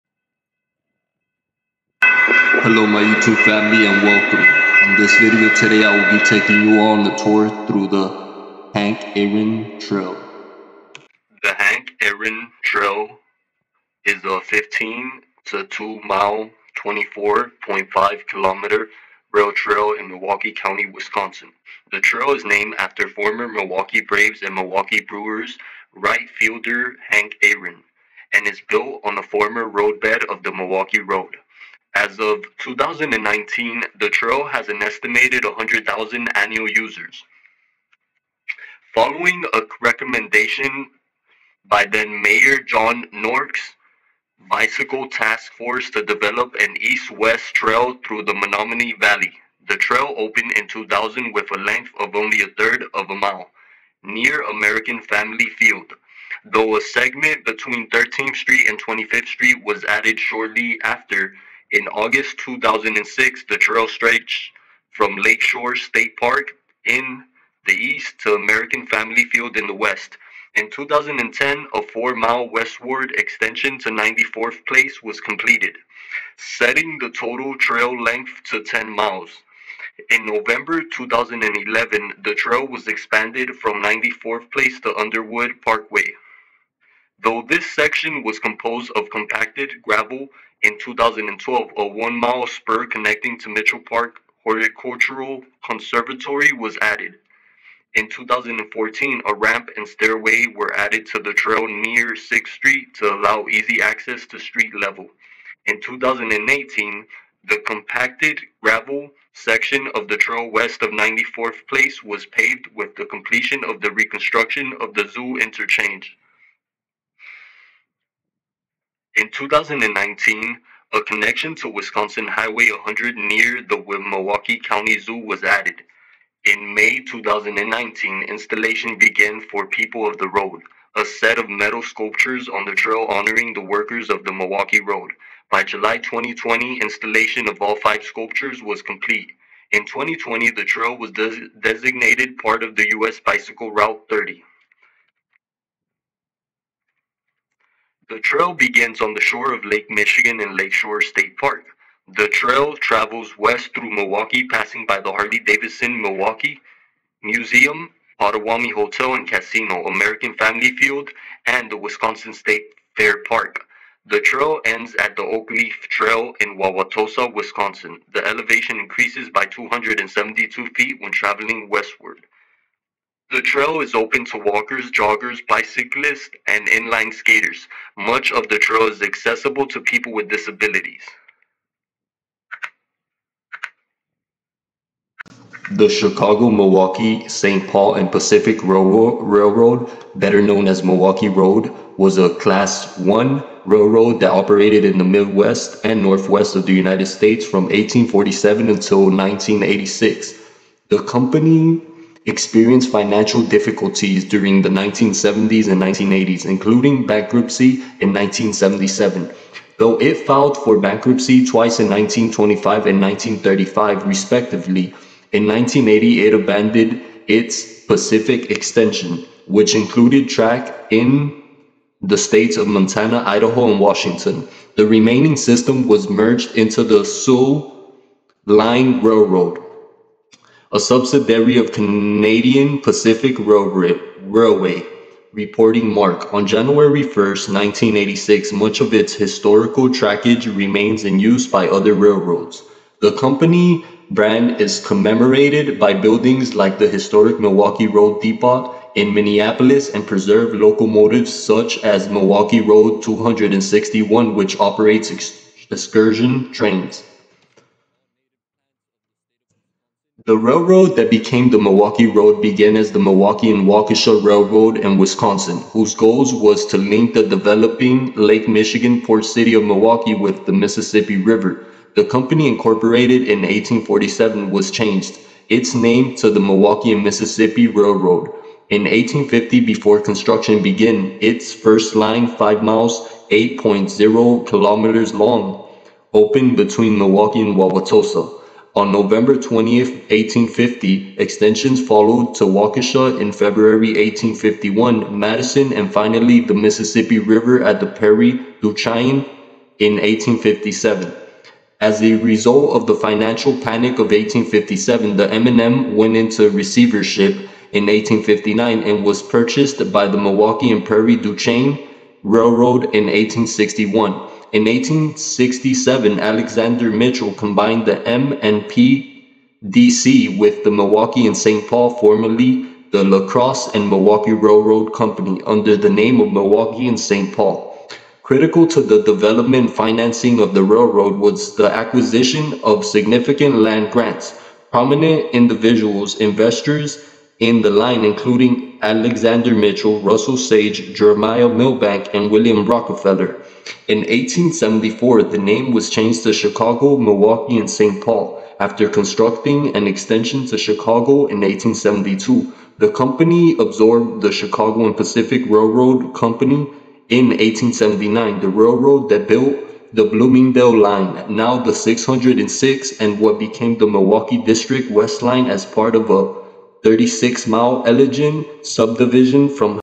hank Aaron State trail
HELLO EVERYONE IN THIS VIDEO ILL BE EXPLORING THE HISTORY OF THE GREAT HANK AARON STATE TRAIL TOUR